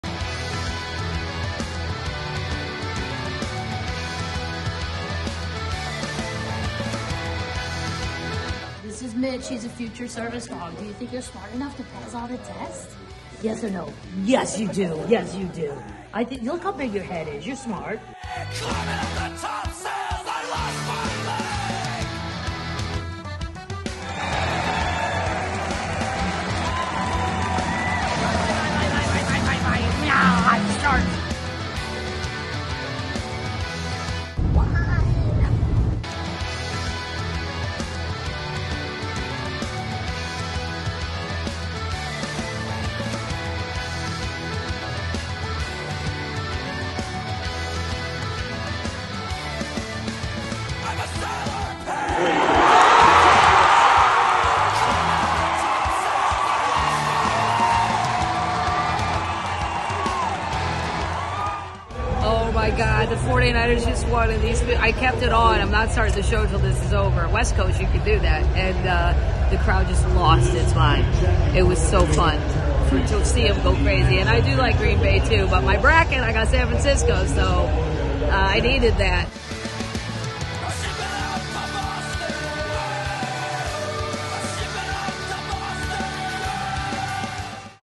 Weekend Review | Santa Rosa & Sacramento
SO much fun last weekend in Santa Rosa at the Luther Burbank Center for the Arts & Wheatland at the Hard Rock Casino 🙌🏻

I’m still rolling on the West coast! See you California Termites this weekend in San Luis Obispo + Monterey🎉.